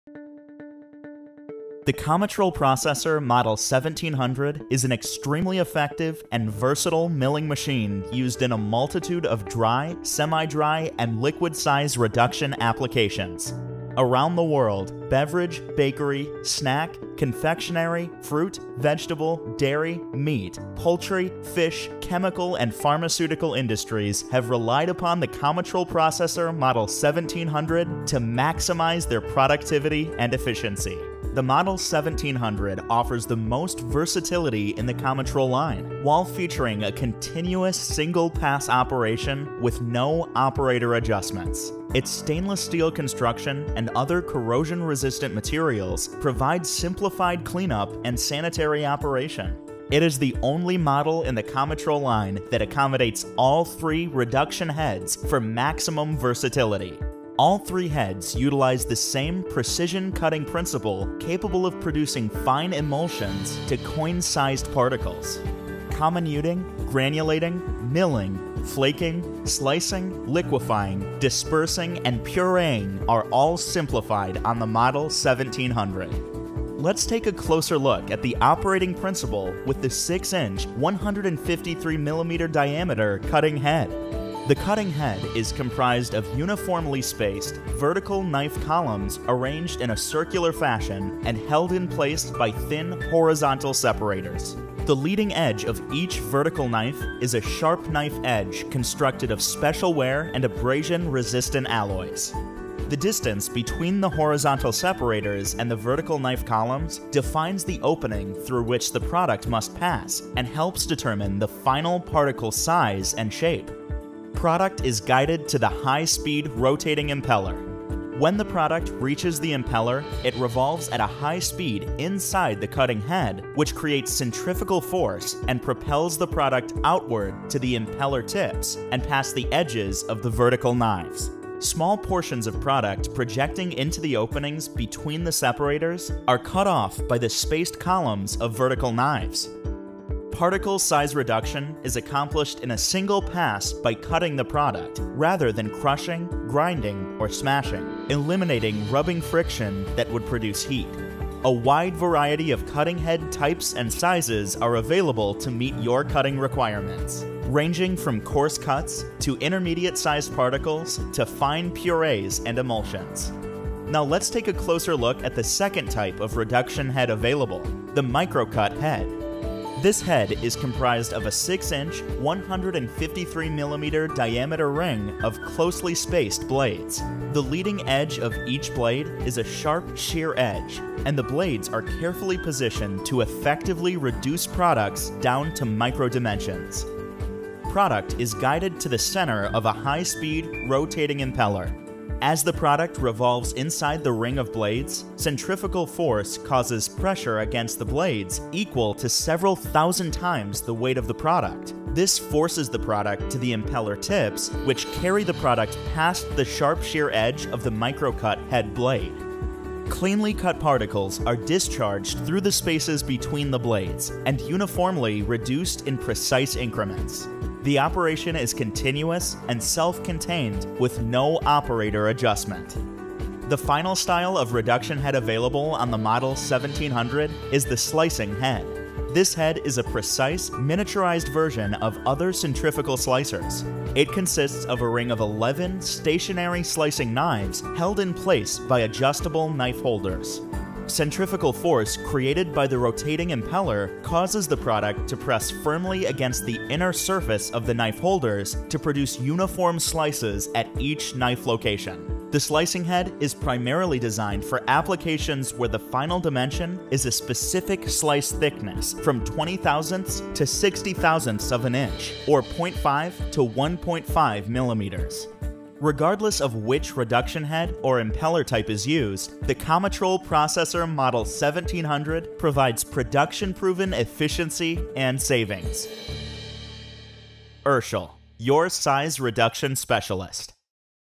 Comitrol Processor Model 1700 Animation
Comitrol Processor Model 1700 machine animation detailing the features and options available.